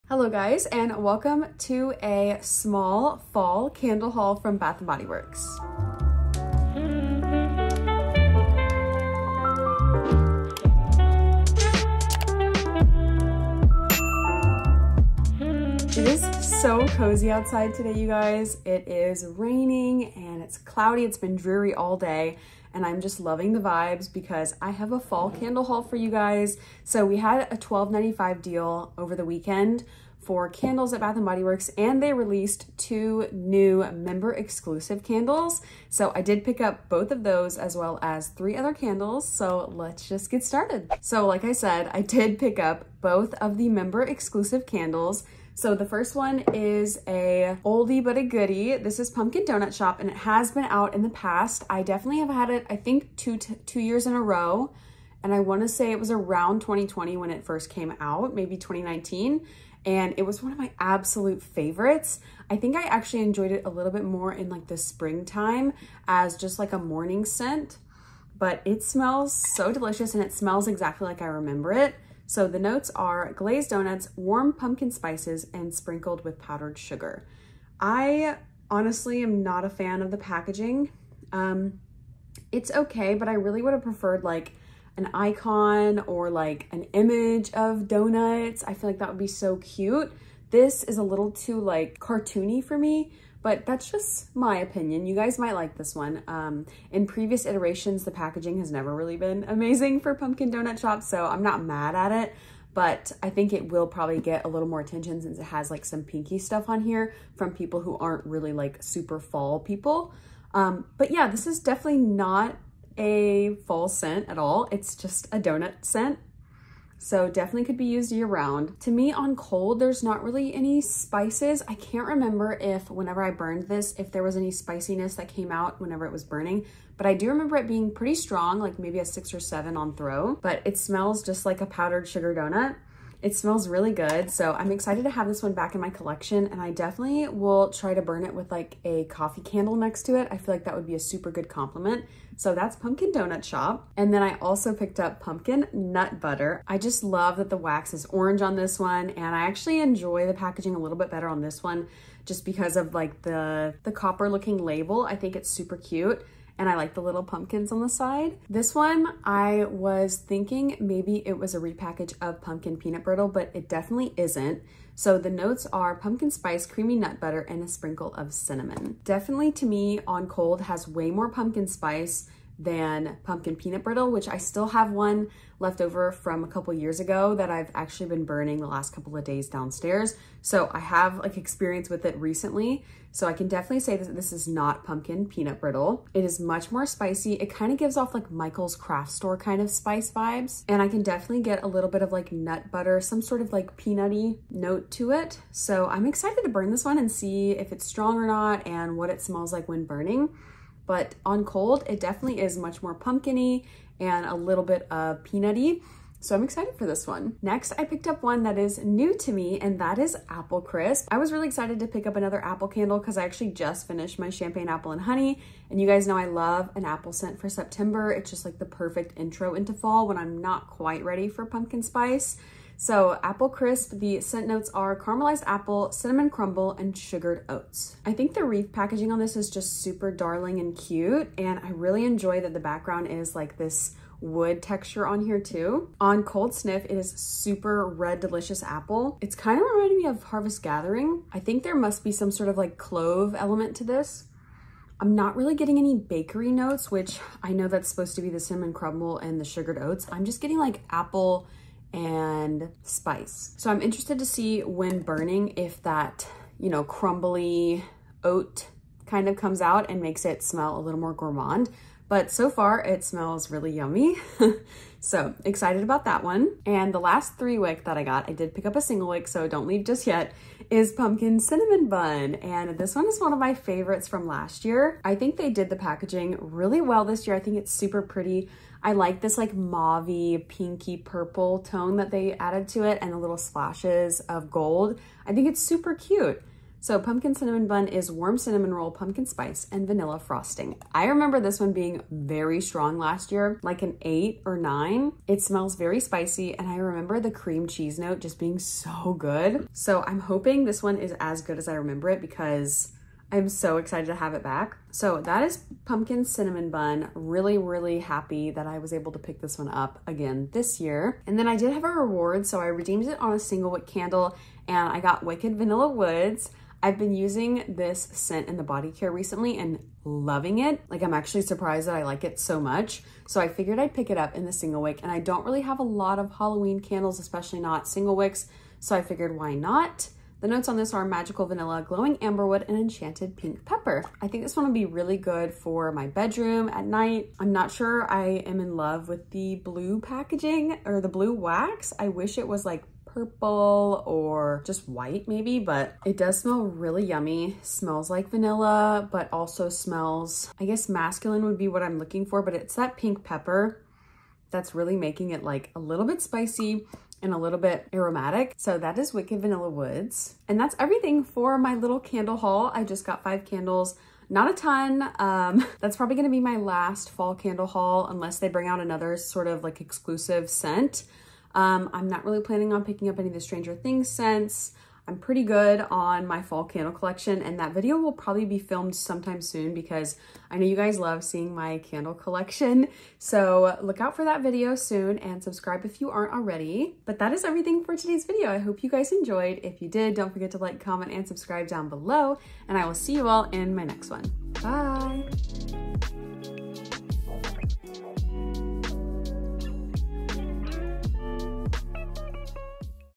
🍂FALL CANDLE HAUL | Bath & Body Works Member Exclusive Candles
Hi everybody! Today I’m bringing you a Bath & Body Works candle haul! Hope you enjoy!

Come save money on Fetch with me❤️ Sign up w/ code PAXD1J & get 2,000 pts See you there!

This video is about:
bath and body works, bath and body works haul, fall haul, candle haul, fall aesthetic, fall scents, fall fragrance, pumpkin spice latte